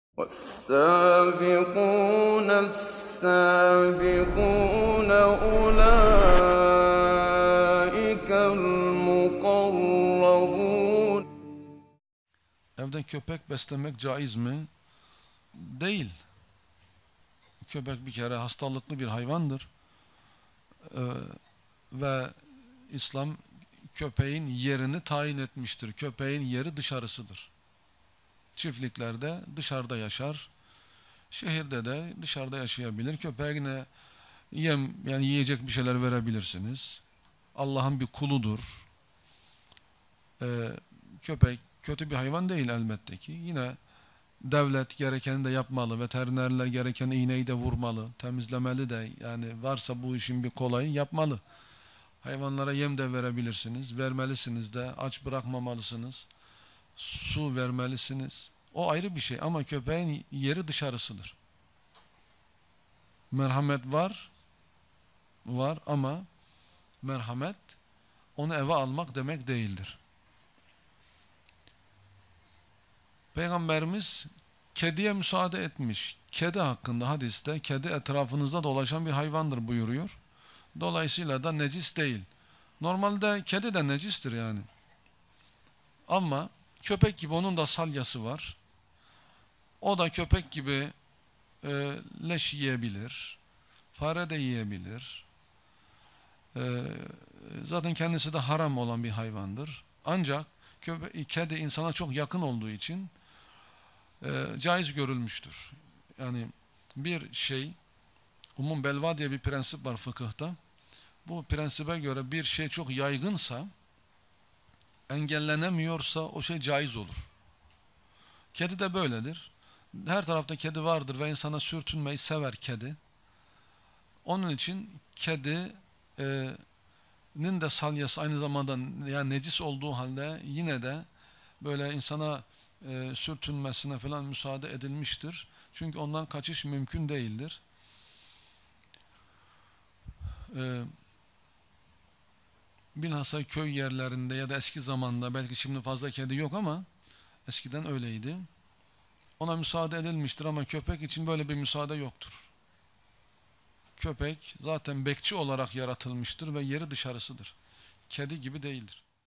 Ist es erlaubt ein Hund als Haustier zu halten? - Alparslan Kuytul Hocaefendi
Hundehaltung im Islam. Ist das erlaubt?
Darf man als Muslim einen Hund in der Wohnung halten?

llllllllllllllllllllllllllllllllllllllllllllllllllllllllllllllllllllllllllllllllllllllllllllllllllllllllllllllllllllllllllll
IHED-Islamischer Humanitärer Entwicklungsdienst